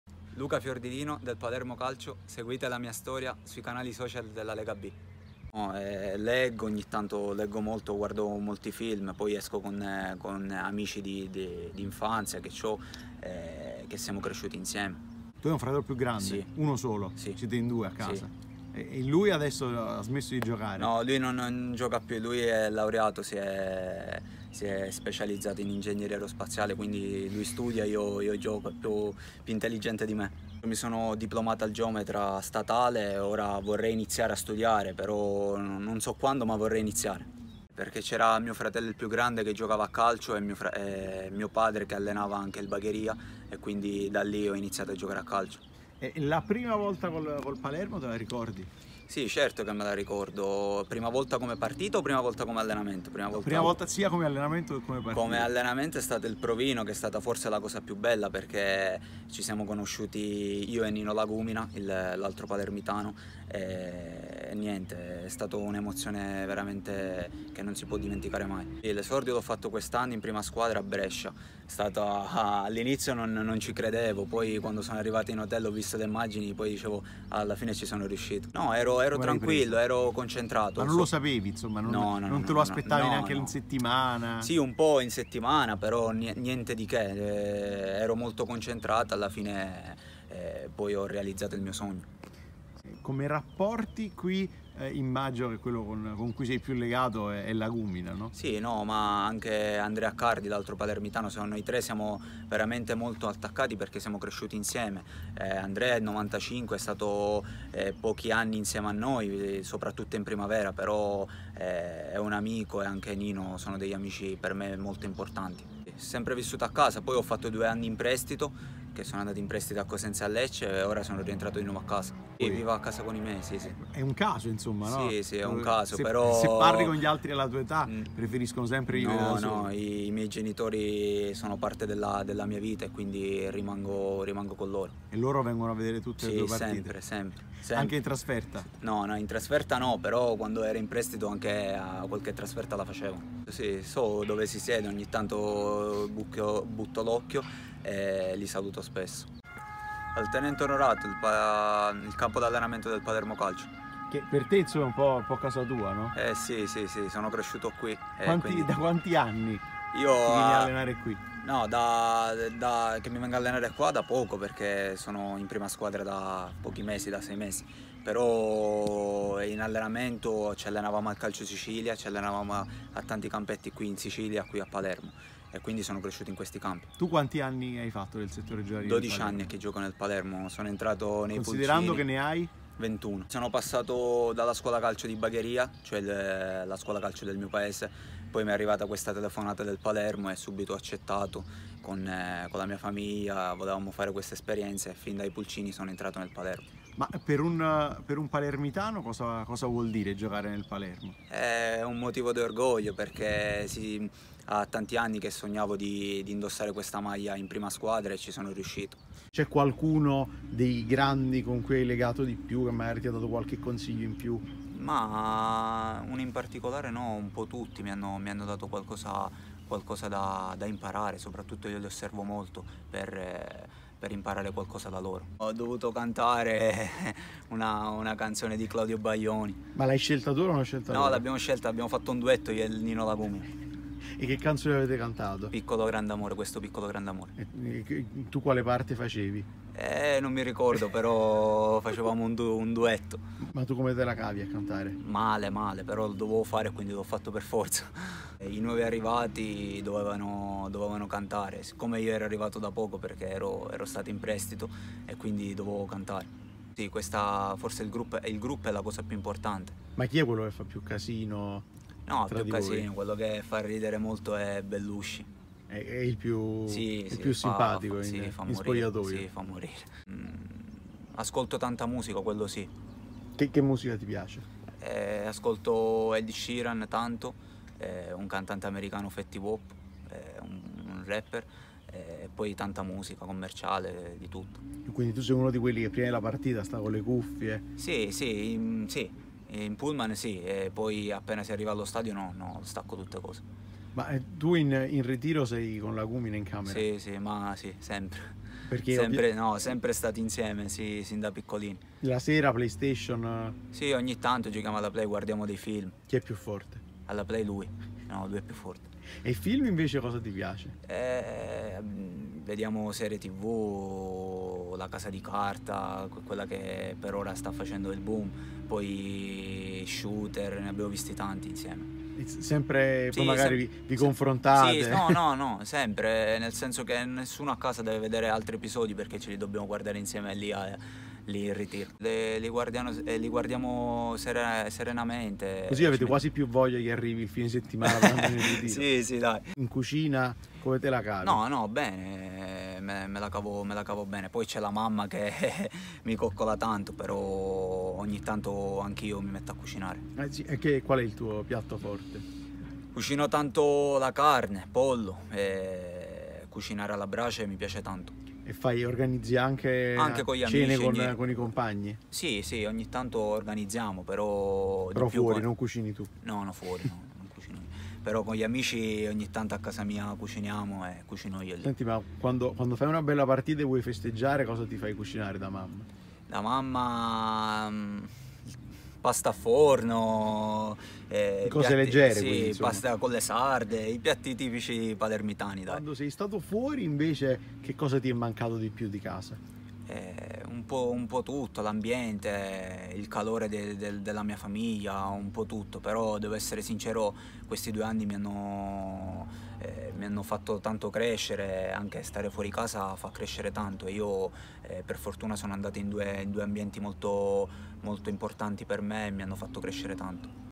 B Marzio con Fiordilino
La rubrica che racconta i migliori talenti della Serie B ConTe.it fa tappa a Palermo per scoprire le qualità di Fiordilino.